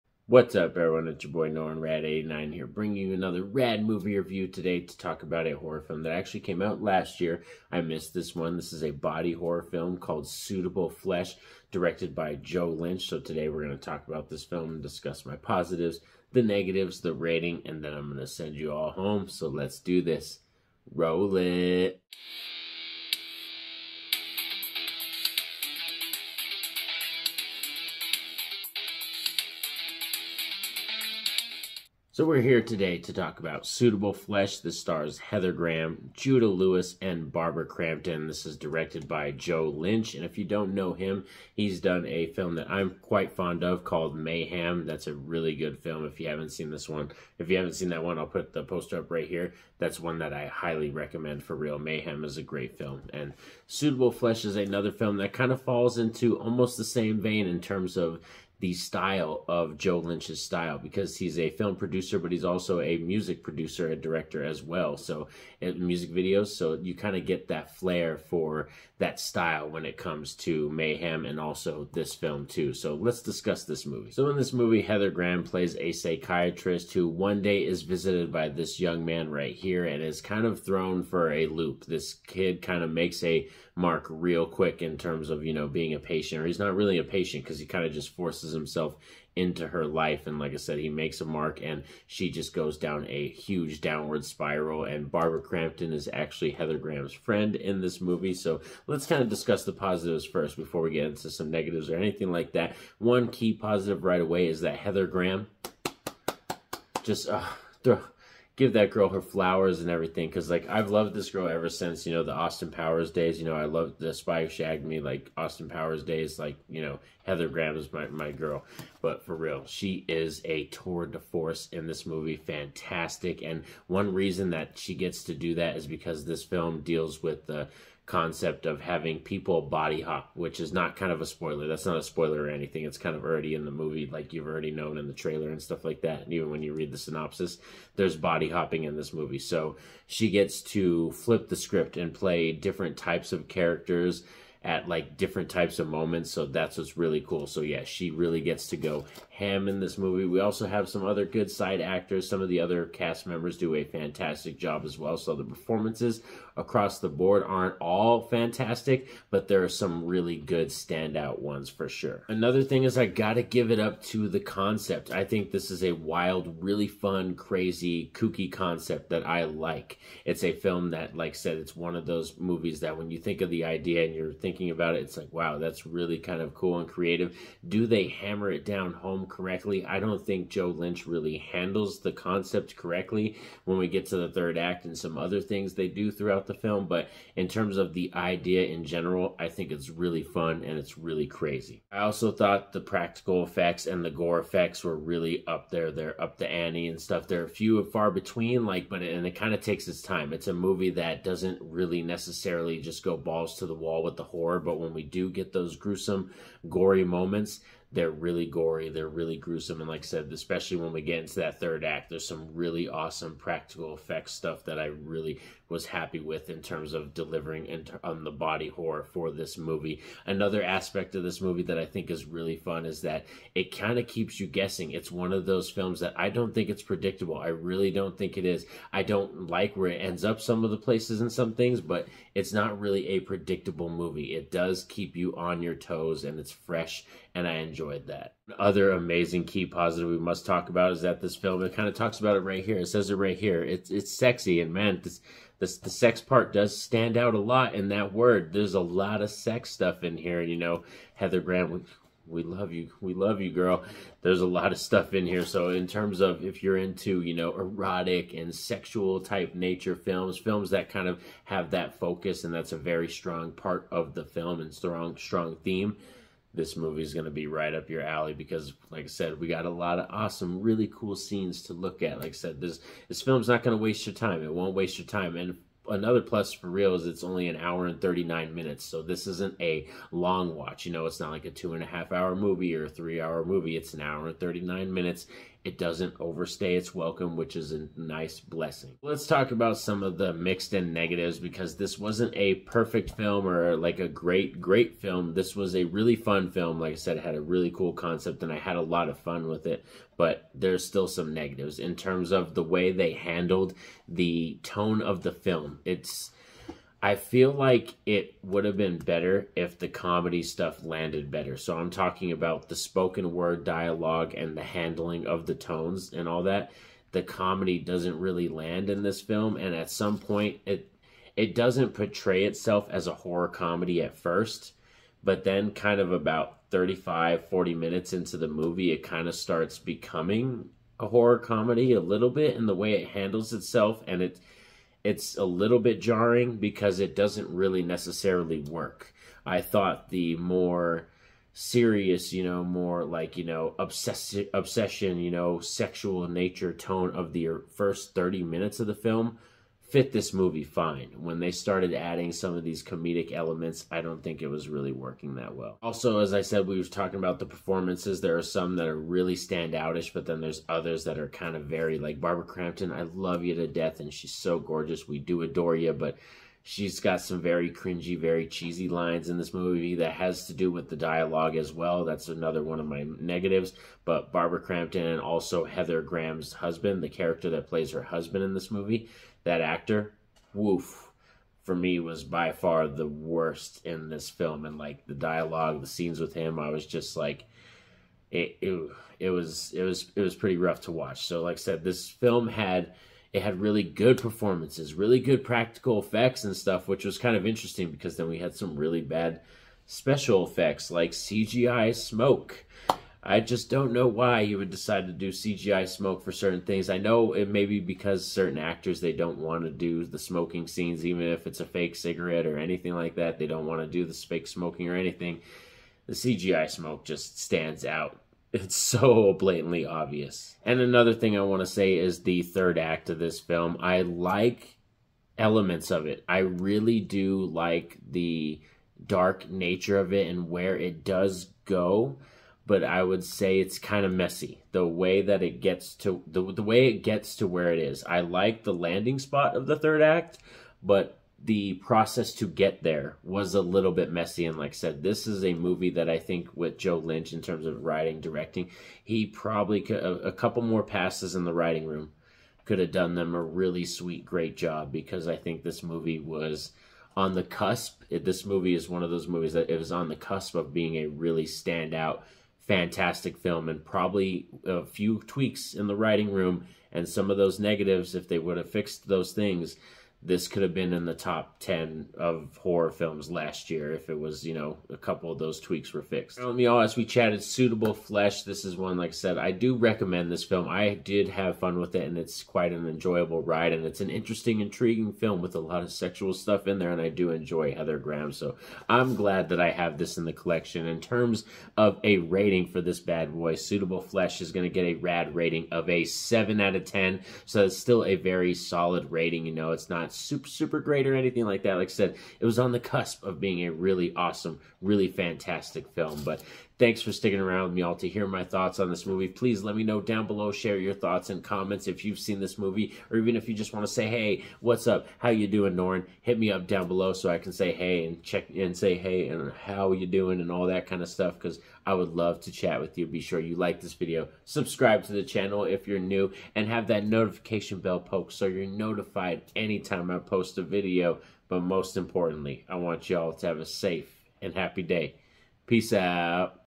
Suitable Flesh Radd Movie Review
Suitable Flesh is a 2023 American horror film written by Dennis Paoli and directed by Joe Lynch. Based on the 1937 H. P. Lovecraft short story "The Thing on the Doorstep", it stars Heather Graham, Judah Lewis, Bruce Davison, Johnathon Schaech and Barbara Crampton.